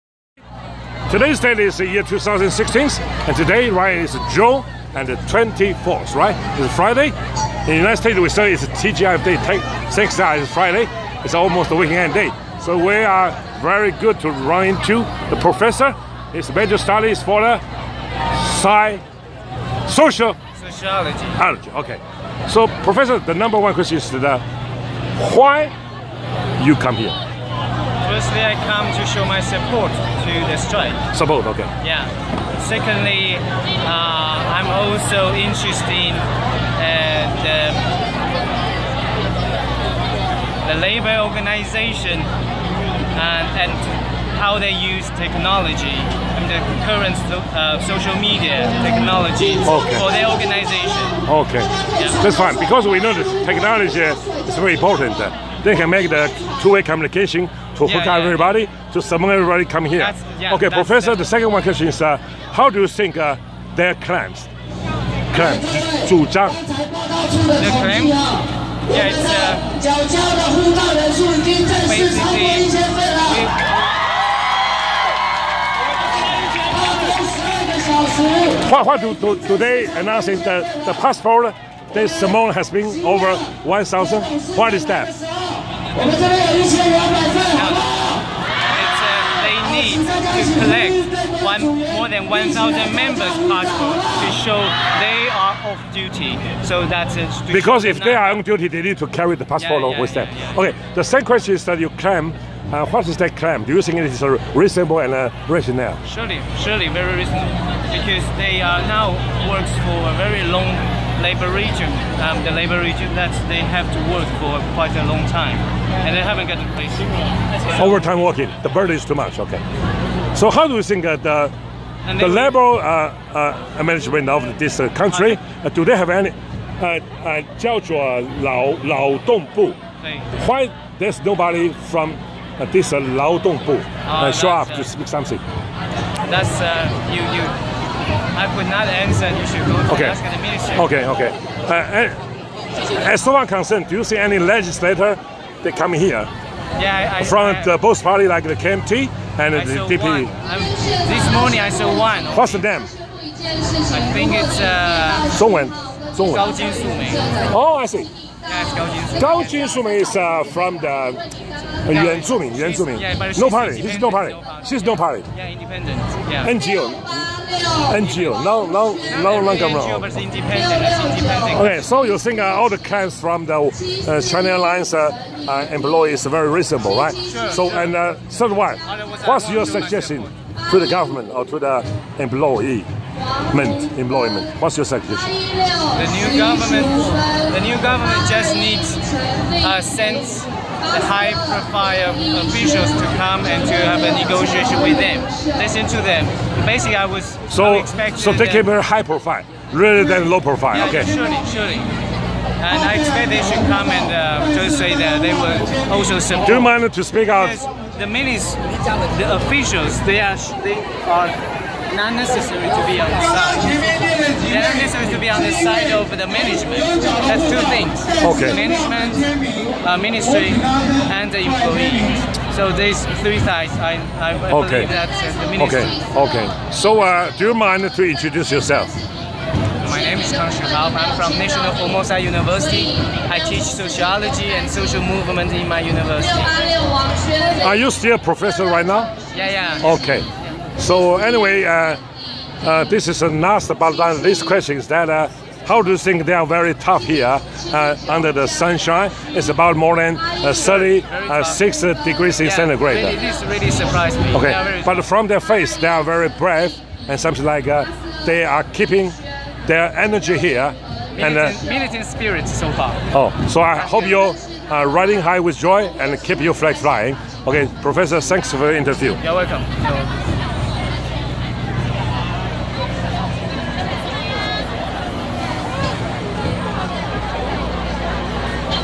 華航罷工現場 , 受訪者 : 國立虎尾科技大學社會學系教授 ;
On the spot of China Airline Strike, ran into Mr. Professor of National Formosa University ;

康世昊 (Shih-Hao Kang) 老師, PhD, University of Warwick社會學系博士 ;
兼任國際事務處境外學生事務組組長 ;
專業領域 : 社會轉型、勞動關係、環境社會學、社會運動、工業民族誌研究方法 ;

Interviewed by : Petertaos, Taiwan Executives,
D.C. Think-tank US S&H Ret. Union